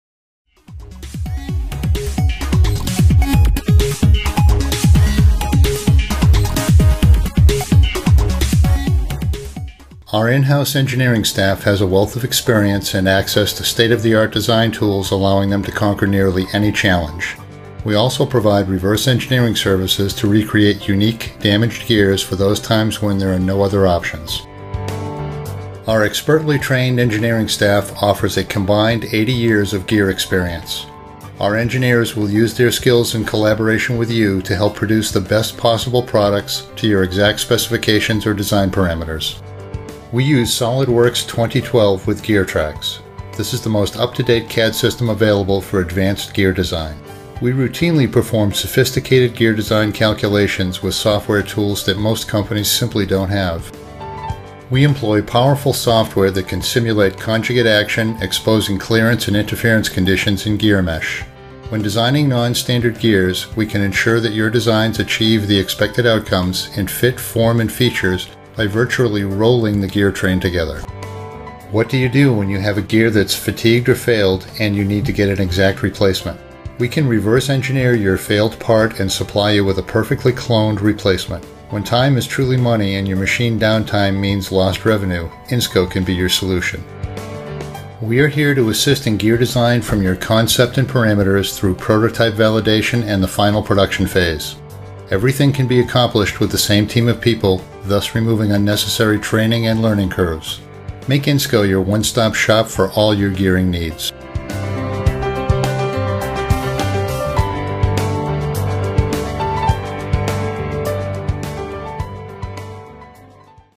Gear Design & Reverse Engineering at Insco Corporation
| Compete, state-of-the-art gear design, from concept through manufacture. Our leading edge solid modeling and involute gear mesh capabilities set as apart as a premier gear manufacturer. We also offer a reverse engineering service form recreating damaged and difficult to replace gears.